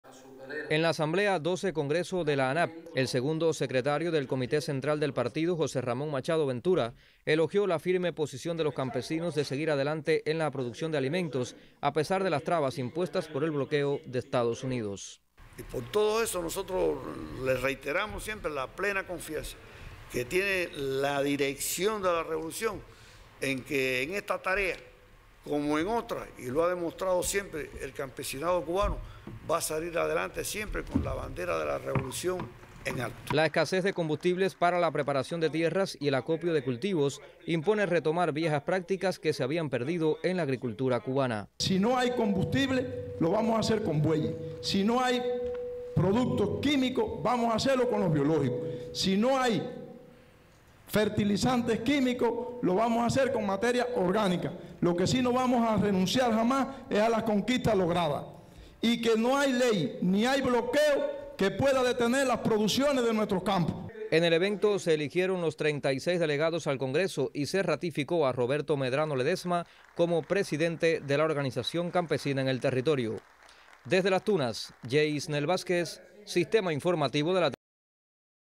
Preside Machado Ventura Asamblea provincial XII Congreso de la ANAP
El Segundo Secretario del Comité Central del Partido Comunista de Cuba, José Ramón Machado Ventura, presidió en Las Tunas la Asamblea Provincial XII Congreso de la Asociación Nacional de Agricultores Pequeños (ANAP). En la reunión, los campesinos tuneros ratificaron su compromiso con la producción de alimentos y la sustitución de importaciones.